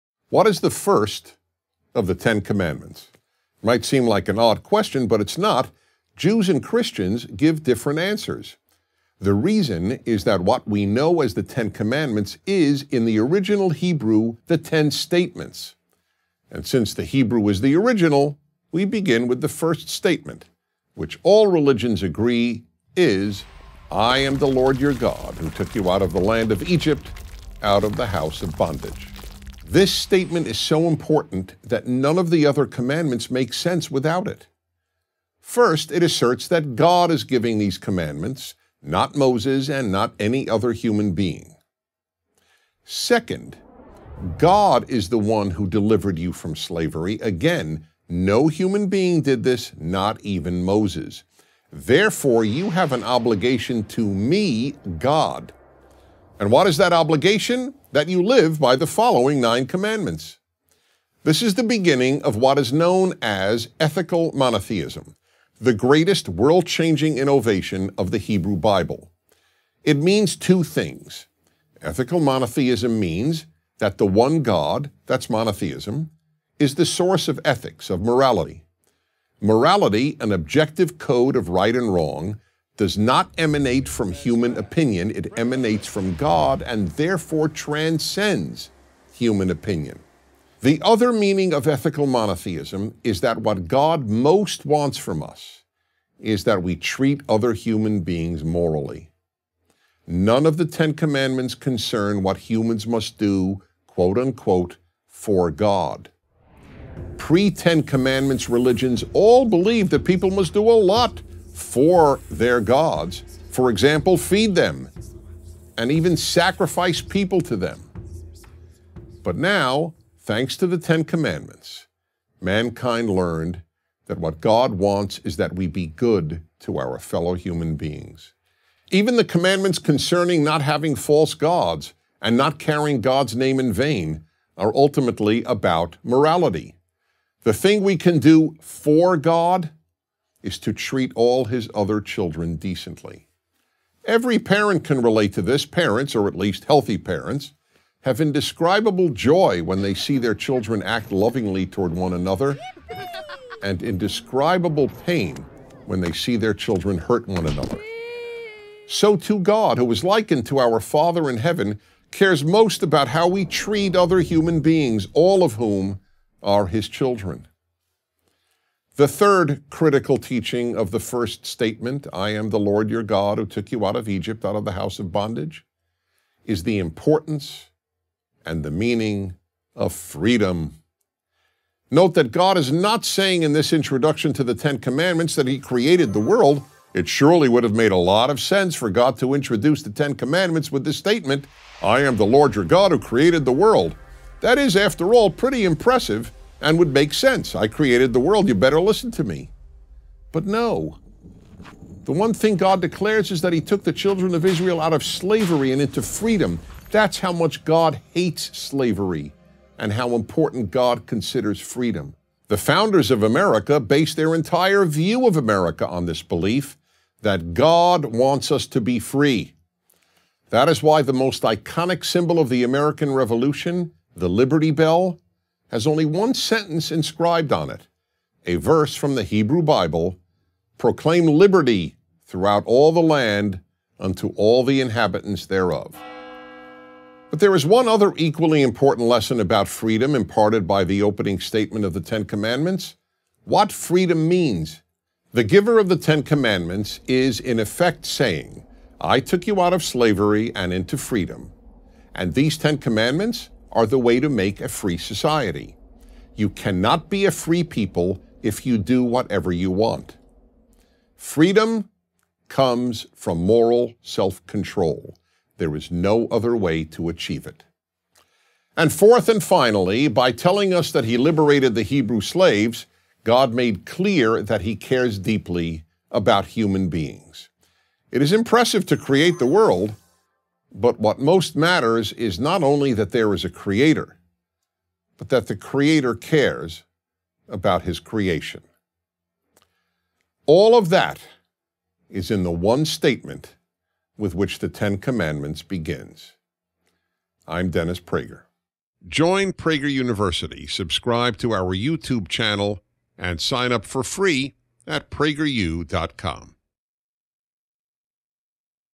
1. I Am the Lord Your God | 5 Minute Video
God Wants Us to Be Free

Although the First Commandment ("I am the Lord your God") appears simple at first glance, it actually set into motion the most revolutionary idea in human history -- ethical monotheism, the belief that there is one God whose main wish is that people treat each other decently. Dennis Prager explains that without this commandment, the following nine mean little. With it, the Ten Commandments becomes world-changing.

🚨 PragerU is experiencing severe censorship on Big Tech platforms. to watch our videos free from censorship!

SHOP! 🛒 Love PragerU?

Script:

What is the first of the Ten Commandments?

It might seem like an odd question, but it's not. Jews and Christians give different answers.  The reason is that what we know as "The Ten Commandments" is, in the original Hebrew, "The Ten Statements." And since the Hebrew is the original, we begin with the first statement, which all religions agree, is:  "I am the Lord your God who took you out of the land of Egypt, out of the house of bondage."  This statement is so important that none of the other commandments make sense without it.

First, it asserts that God is giving these commandments. Not Moses and not any other human being.   Second, God is the One who delivered you from slavery. Again, no human being did this, not even Moses. Therefore you have an obligation to me, God.   And what is that obligation? That you live by the following nine commandments.

This is the beginning of what is known as ethical monotheism, the greatest world-changing innovation of the Hebrew Bible.  It means two things.  Ethical monotheism means that the one God -- that's monotheism -- is the source of ethics, of morality. Morality, an objective code of right and wrong, does not emanate from human opinion; it emanates from God, and therefore transcends human opinion.   The other meaning of ethical monotheism is that what God most wants from us is that we treat other human beings morally. None of the Ten Commandments concern what humans must do "for" God; pre-Ten Commandments religions all believed that people must do a lot "for" their gods -- for example, feed them and even sacrifice people to them.

But now, thanks to the Ten Commandments, mankind learned that what God wants is that we be good to our fellow human beings. Even the commandments concerning not having false gods and not carrying God's name in vain are ultimately about morality.  The thing we can do "for" God is to treat all his other children decently. Every parent can relate to this. Parents -- or at least healthy parents -- have indescribable joy when they see their children act lovingly toward one another and indescribable pain when they see their children hurt one another. So, too, God, who is likened to our father in heaven, cares most about how we treat other human beings, all of whom are His children.

The third critical teaching of the First Statement -- "I am the Lord your God who took you of Egypt, out of the House of bondage" -- is the importance, and the meaning, of freedom.